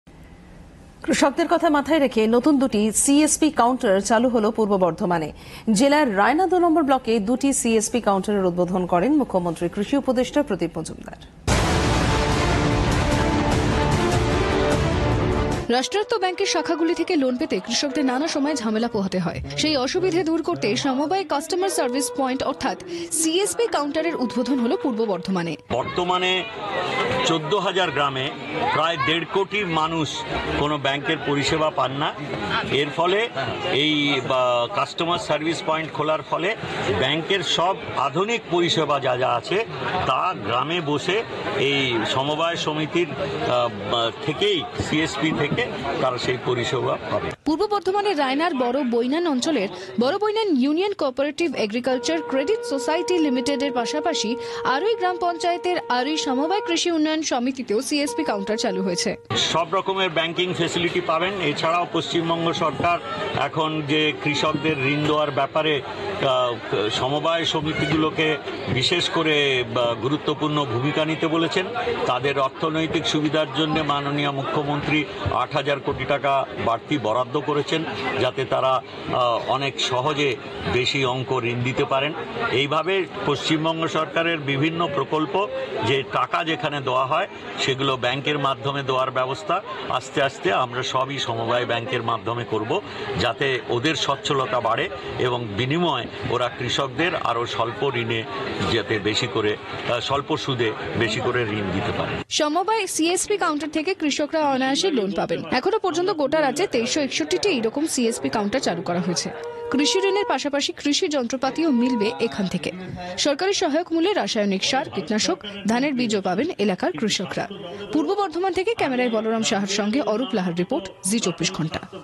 Egiye Bangla: New csp counter at east Bardhaman 'samabay bank' by government initiative
সরকারি উদ্যোগে সমবায় ব্যাঙ্কে নতুন csp কাউন্টার পূর্ব বর্ধমানে , সুবিধা পাবে কৃষকরা